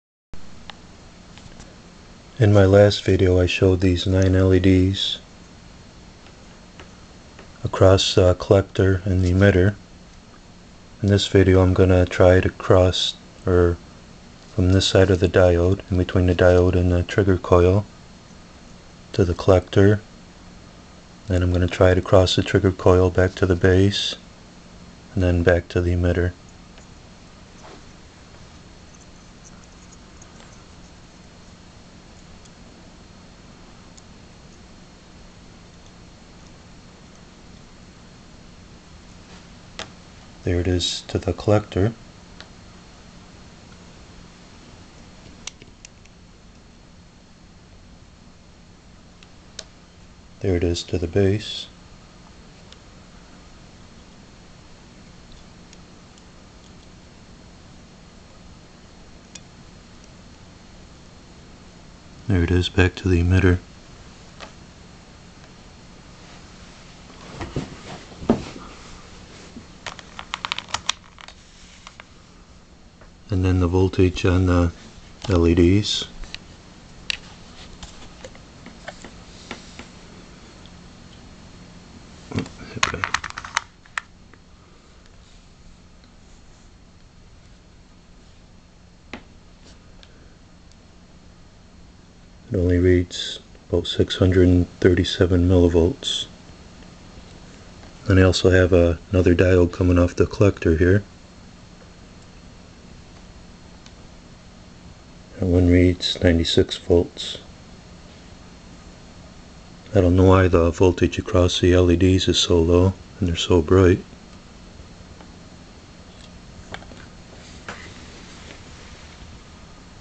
JT Exciter Load Points
In my last video I had the leds across the collector and emitter.
In this video I will show the positive side of the leds in between the diode and trigger coil.
And then the negative side going to the collector the base and then the emitter.